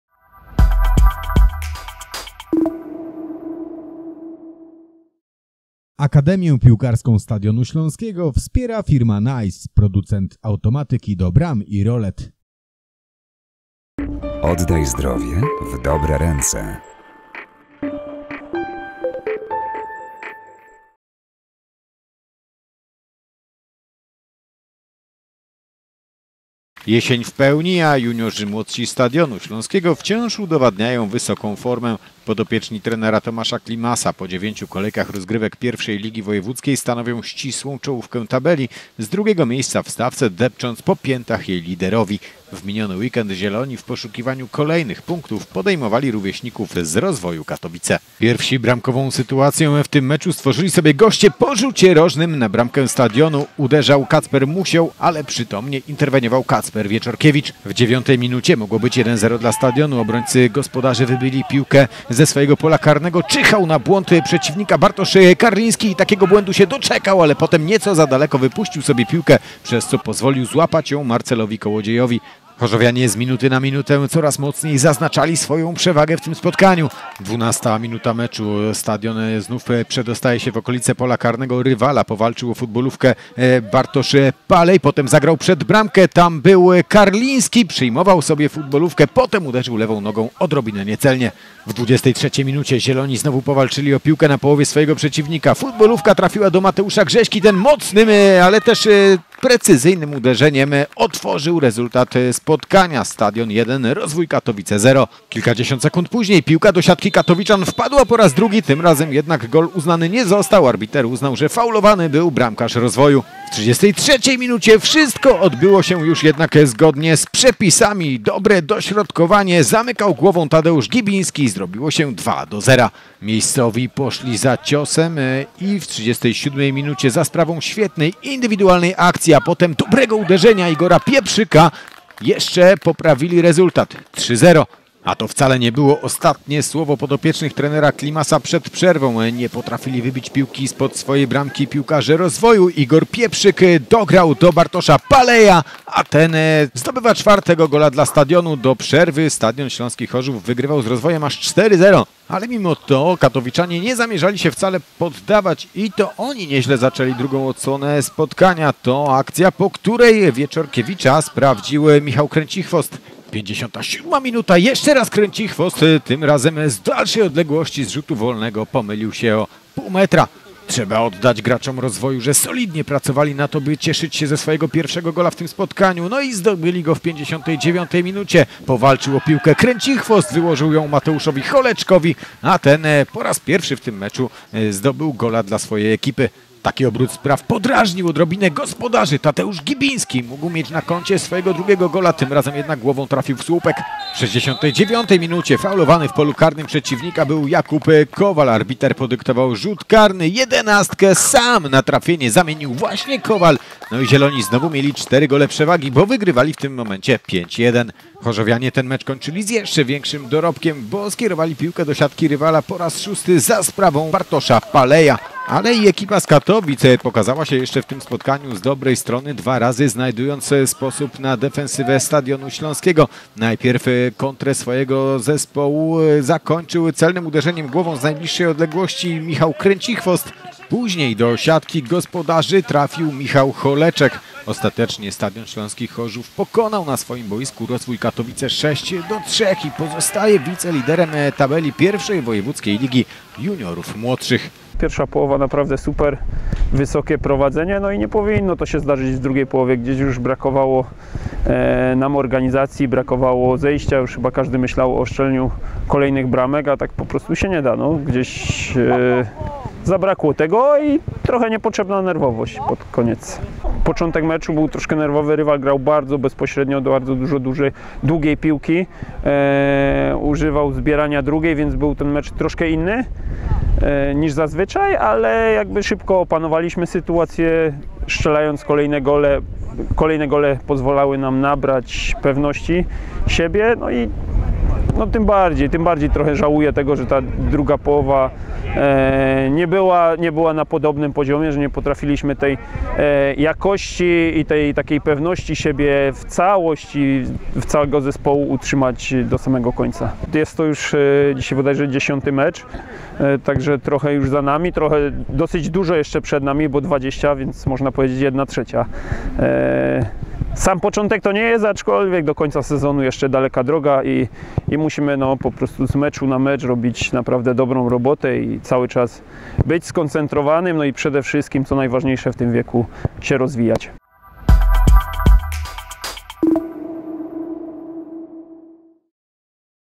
Juniorzy młodsi: Stadion Śląski Chorzów - Rozwój Katowice
Jesień w pełni, a juniorzy młodsi Stadionu Śląskiego wciąż udowadniają wysoką formę. Podopieczni trenera Tomasza Klimasa po dziewięciu kolejkach rozgrywek I Ligi Wojewódzkiej stanową ścisłą czołówkę tabeli, z drugiego miejsca w stawce depcząc po piętach jej liderowi. W miniony weekend "Zieloni" zdobyli kolejne punkty, wygrywając z rówieśnikami z Rozwoju Katowice.